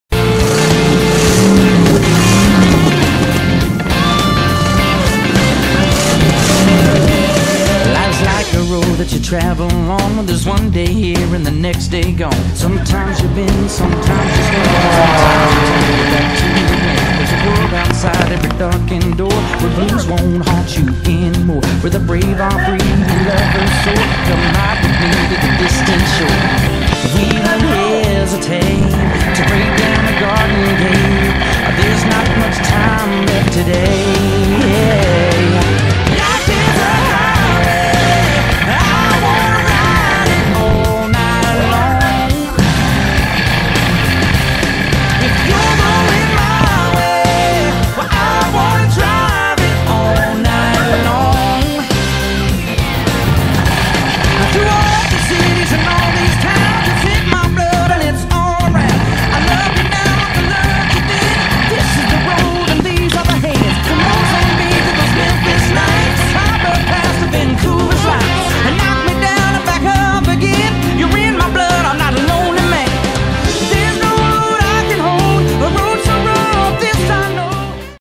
Pinewood Derby 2017
Just a quick video of our cub scout pinewood derby races.